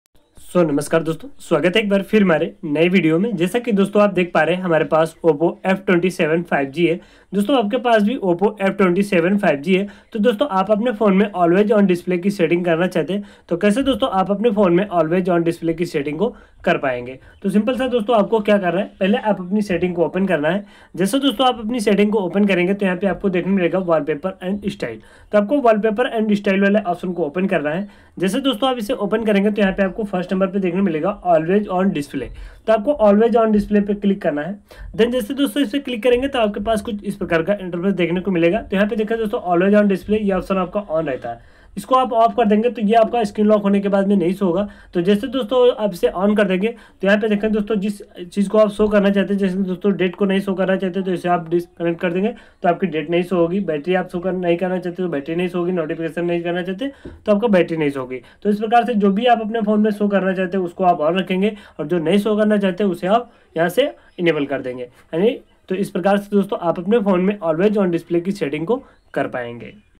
oppo f27 5g always on display setting | how to enable always on display in oppo f27 5g
Doston aaj ke is video mein humne aapko bataya ki oppo f27 5g me always on display setting kaise karte hai